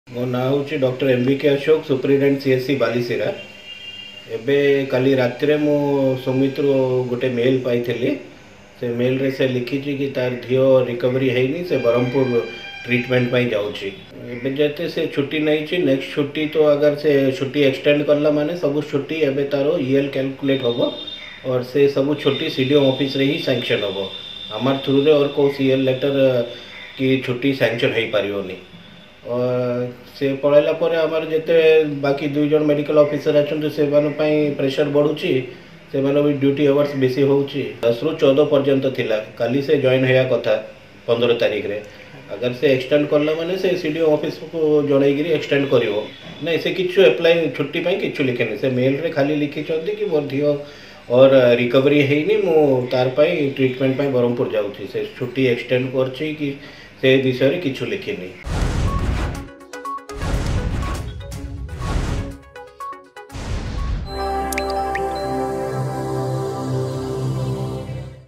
୧୫ ତାରିଖରେ ସୁମିତ ଡ୍ୟୁଟିରେ ଯୋଗଦେବ ତ !
୧୫ ତାରିଖରେ ସୁମିତ ଡ୍ୟୁଟିରେ ଯୋଗଦେବ ତ ! ଶୁଣନ୍ତୁ ଏ ନେଇ ବାଲିଶିରାର ଡାକ୍ତର କ'ଣ କହୁଛନ୍ତି